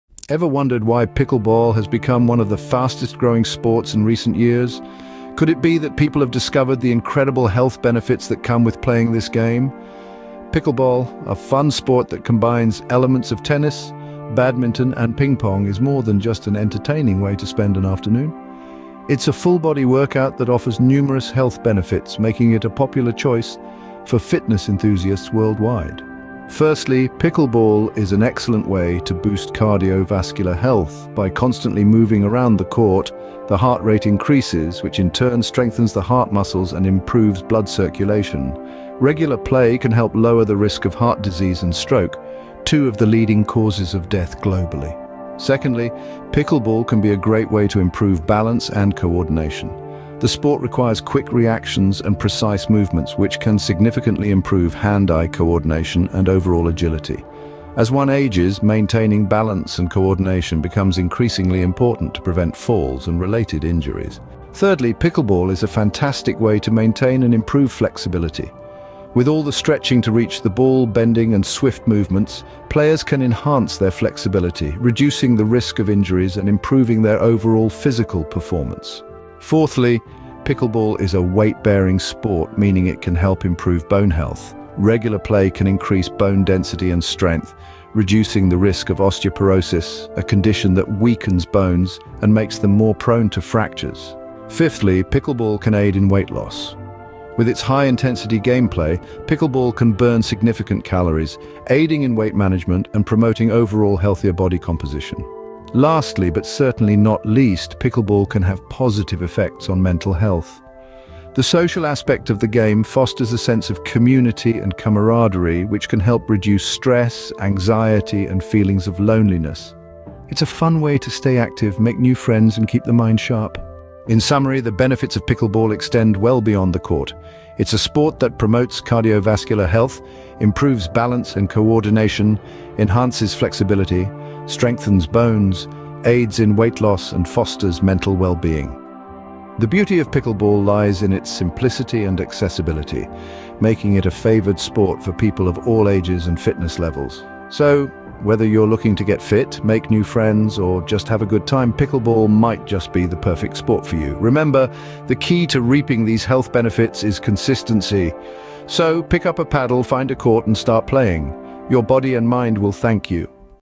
Pickleball The Game Changer for Your Body
00:00:00
Introduction to Pickleball

00:00:26
Cardiovascular Health

00:00:45
Balance and Coordination

00:01:04
Flexibility

00:01:19
Bone Health

00:01:34
Weight Loss

00:01:45
Mental Health

00:02:03